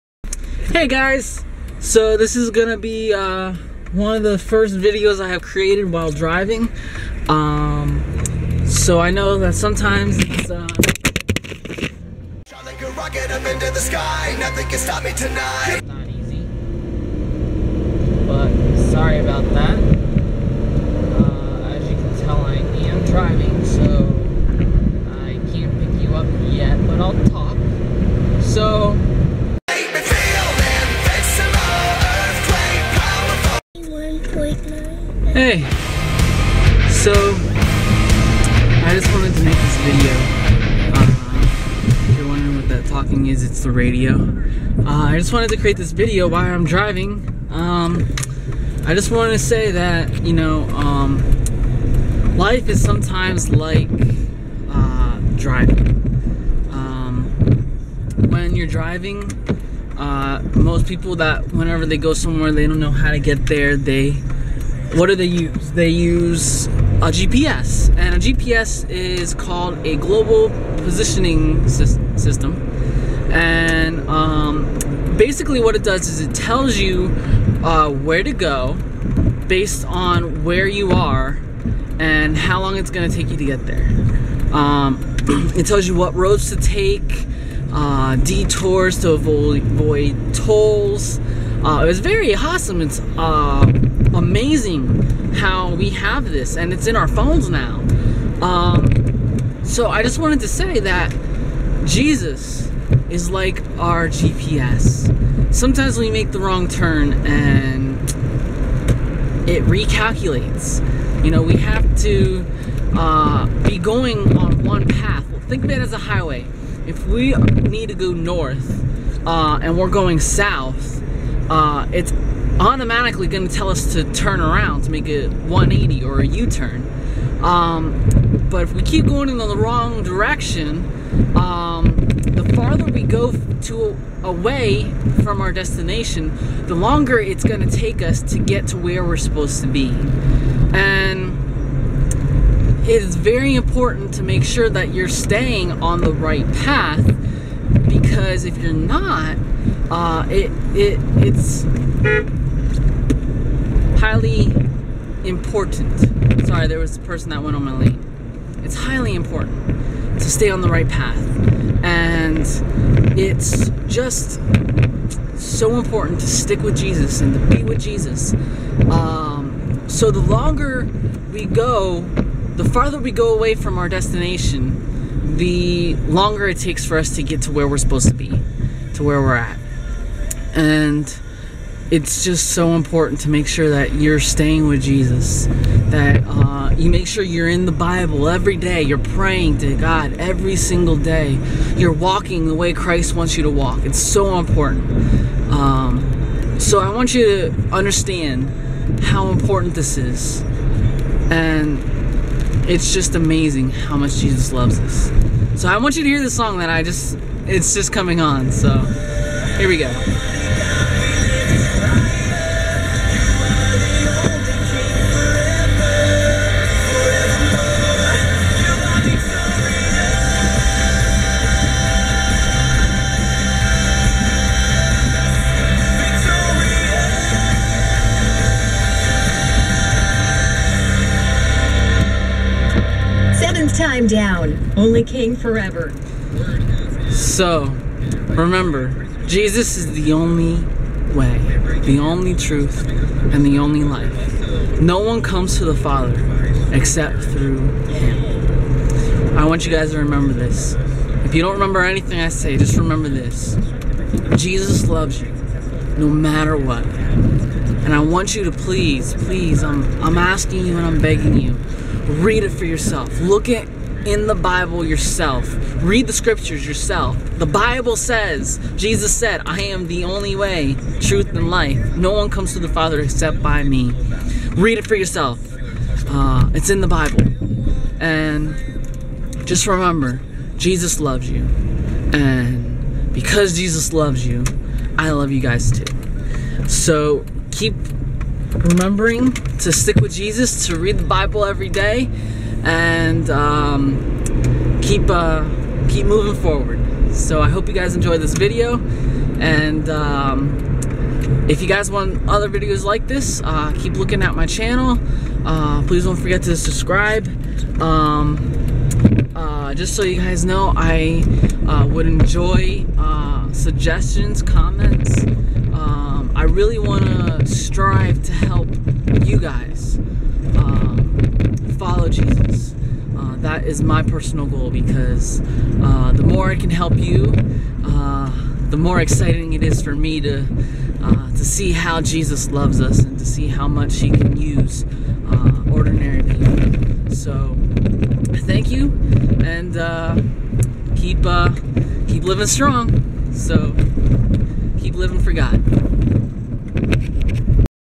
First car video.|Beginning is Epicfail
This is my first video inside a car and driving, watch how the beginning becomes a FAIL! And I try to explain!